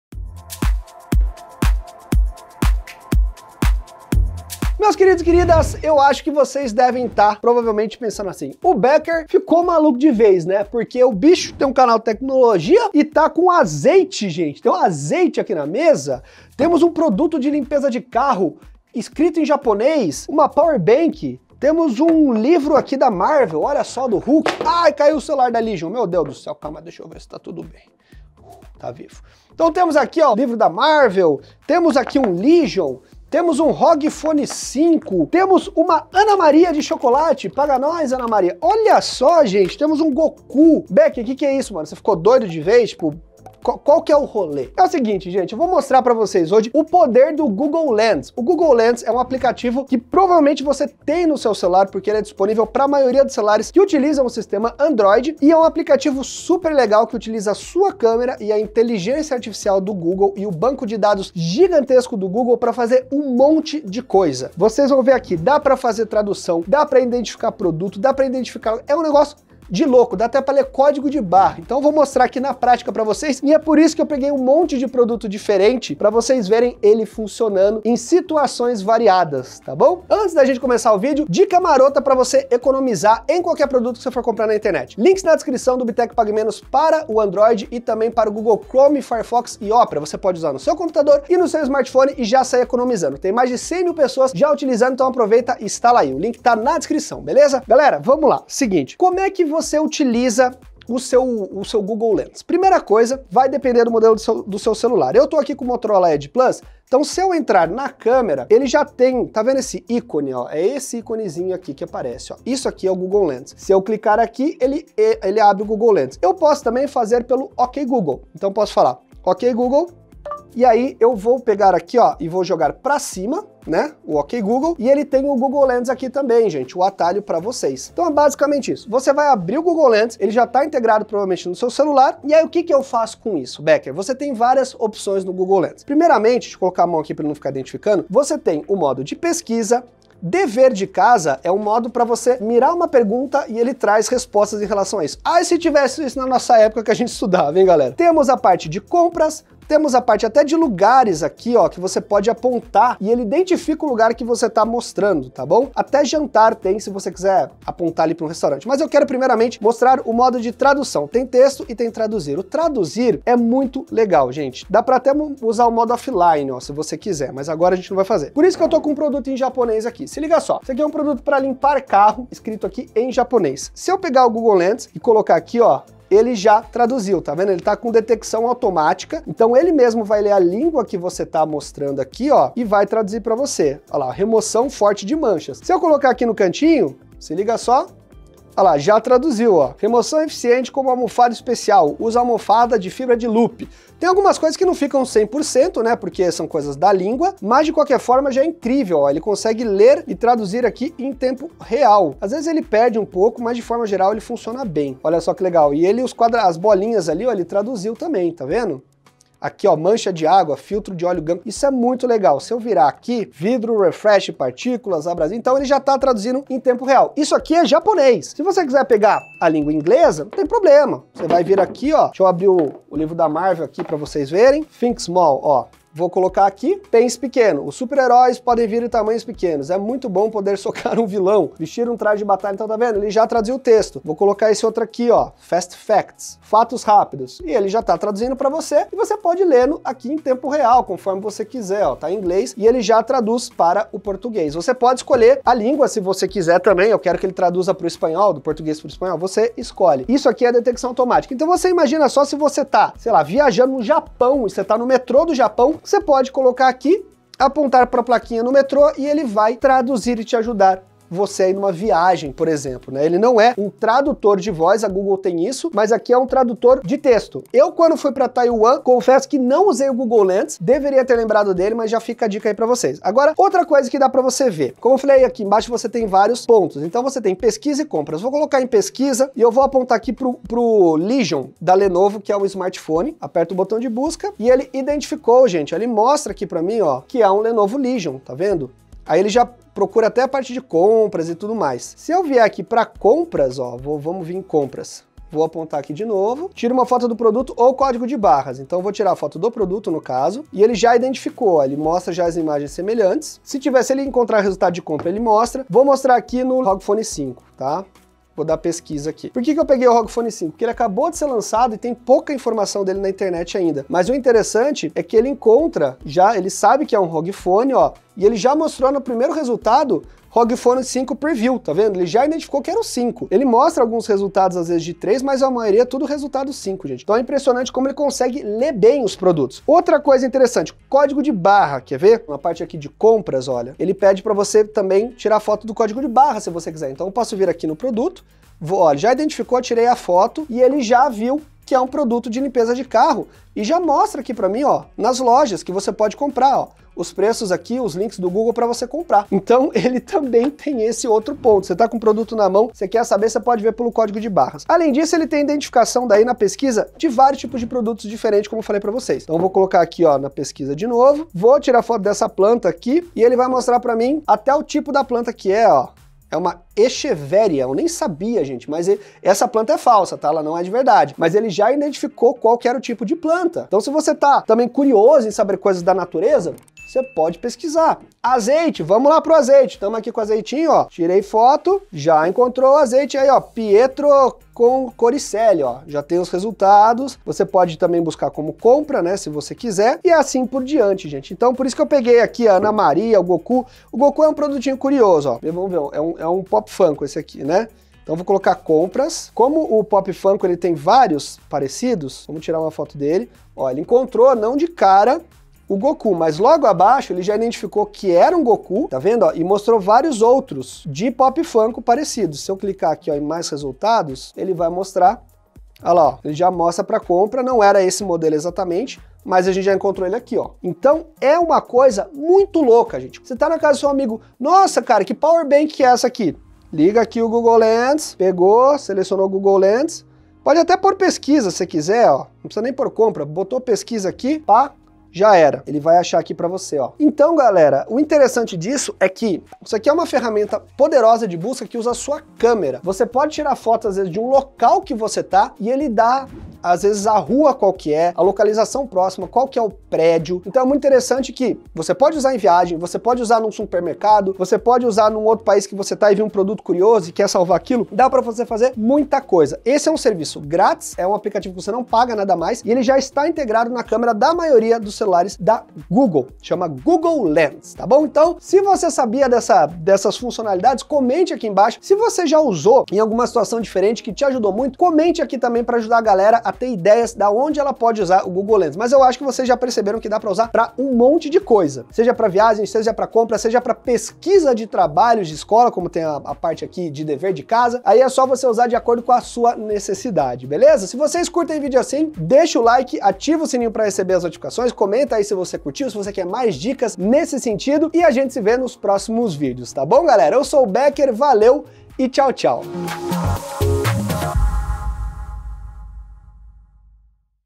VOCÊ tem ISSO no seu SMARTPHONE e NEM SABE o PODER DELE! GOOGLE LENS! Incrível e PRÁTICO demais!
🔴Participe do nosso GRUPO de OFERTAS!

Muita gente já ouvi falar e possuem este aplicativo no próprio smartphone, mas desconhecem o quão bom e prático ele pode ser! O Google Lens é grátis e pode te ajudar em várias situações! Bora conferir!